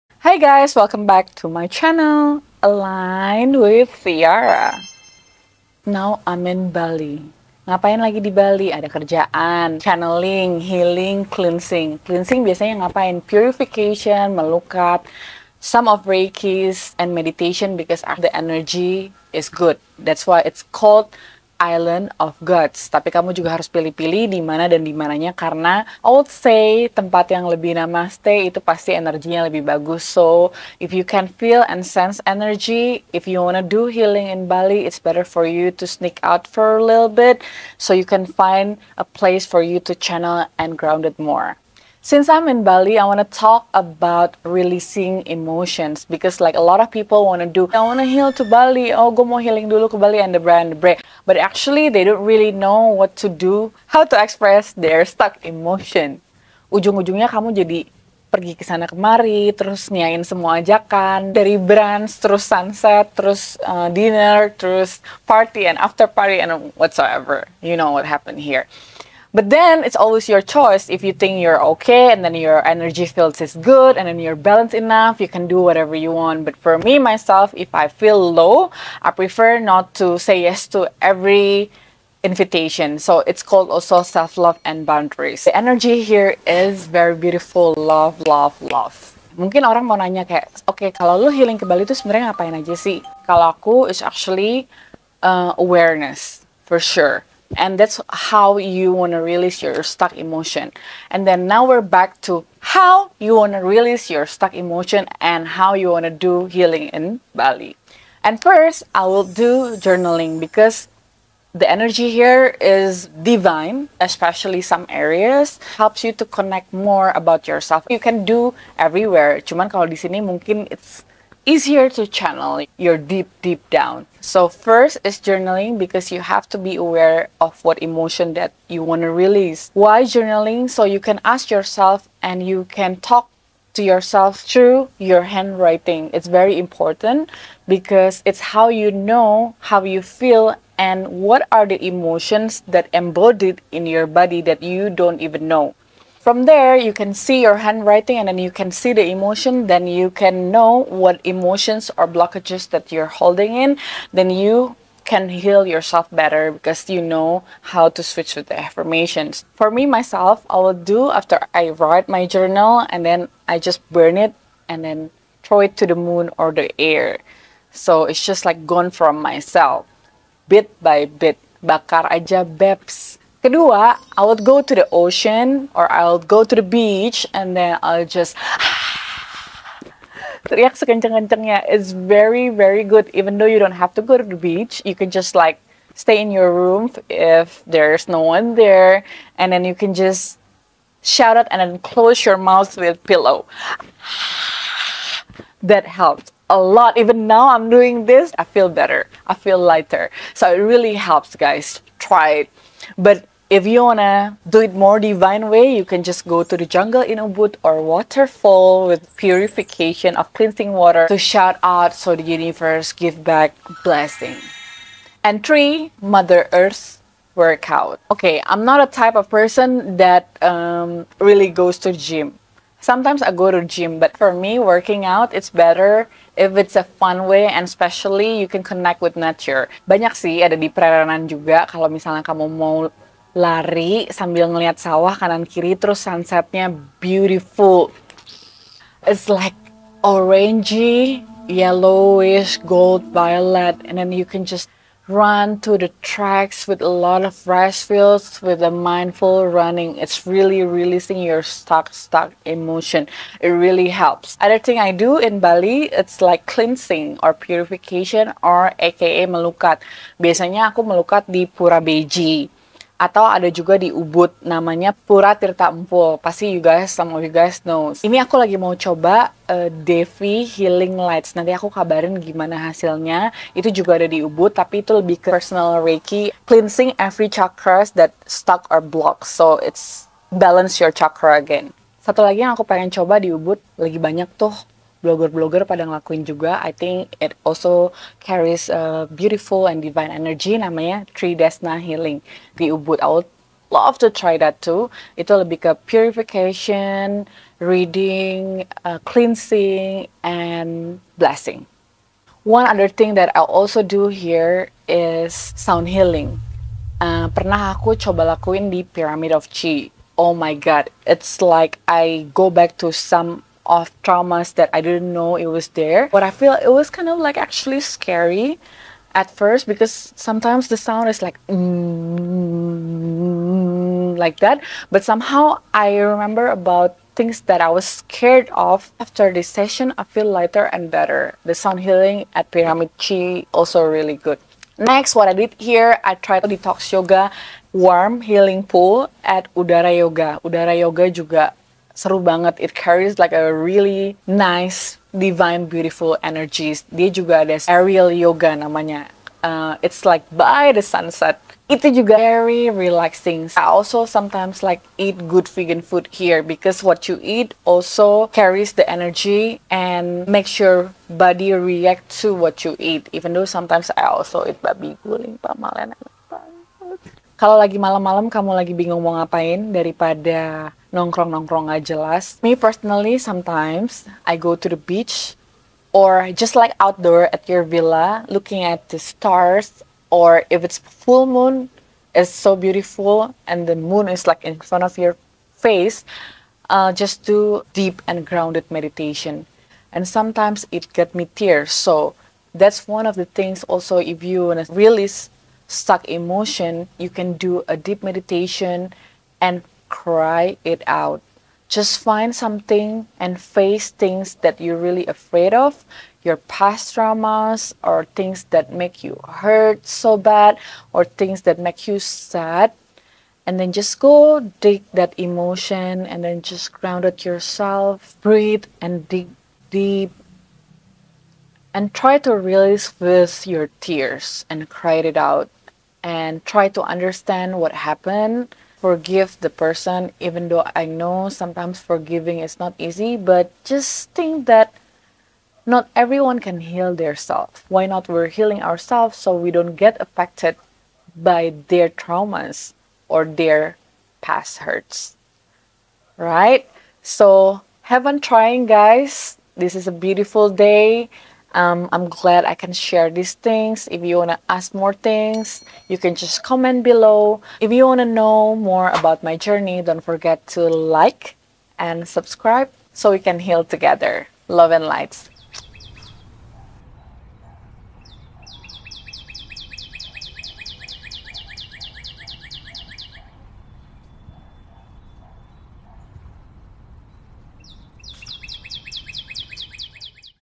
3 WAYS TO RELEASE YOUR EMOTIONS IN A HEALTHY WAY | SOME RECOMMENDATIONS TO DO HEALING IN BALI
Ways to release stuck emotions in healthy way. Since i'm in Bali at the moment, i also shared some of healing things i do in Bali, nevertheless you can also try it in anywhere that relates with step.

Have fun trying, love & lights✨💖